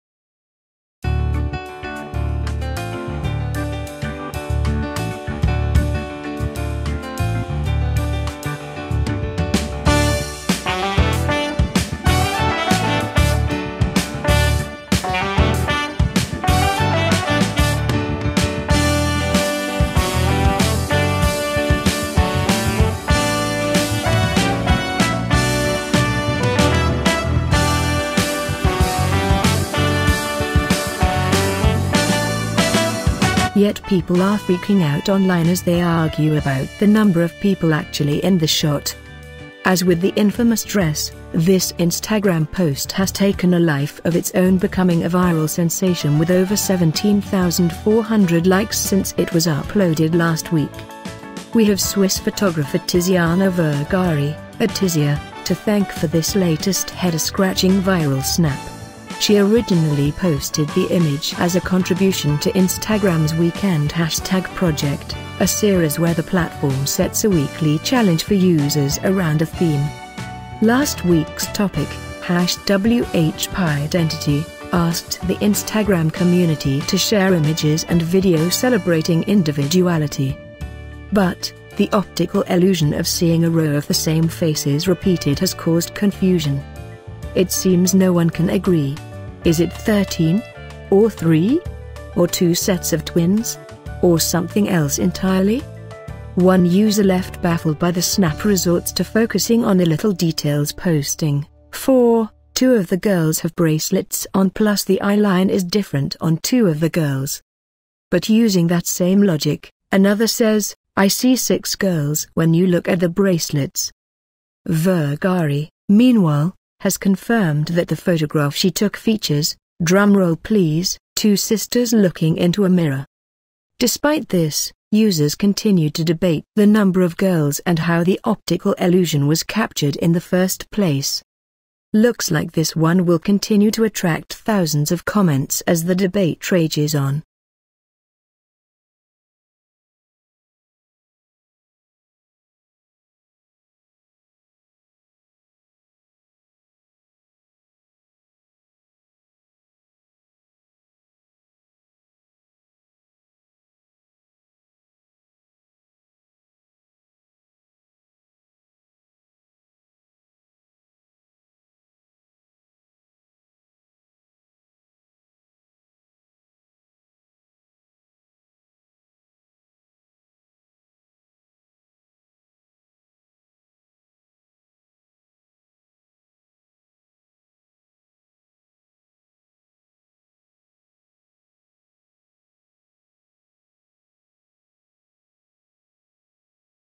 The Internet can't work out how many girls are in this Instagram photo
Yet people are freaking out online as they argue about the number of people actually in the shot.

As with the infamous dress, this Instagram post has taken a life of its own becoming a viral sensation with over 17,400 likes since it was uploaded last week.

We have Swiss photographer Tiziana Vergari (@tizzia) to thank for this latest head-scratching viral snap. She originally posted the image as a contribution to Instagram's weekend hashtag project, a series where the platform sets a weekly challenge for users around a theme.

Last week's topic, WHPidentity, asked the Instagram community to share images and video "celebrating individuality."

But, the 'optical illusion' of seeing a row of the same faces repeated has caused confusion.

It seems no one can agree. Is it 13? Or 3? Or 2 sets of twins? Or something else entirely?

One user left baffled by the snap resorts to focusing on the little details posting: "Four, two of the girls have bracelets on plus the eye line is different on two of the girls."

But using that same logic, another says: "I see 6 girls when you look at the bracelets."

Vergari, meanwhile, has confirmed that the photograph she took features -- drum roll please -- two sisters looking into a mirror.

Despite this, users continue to debate the number of girls and how the "optical illusion" was captured in the first place.

Looks like this one will continue to attract thousands of comments as the debate rages on.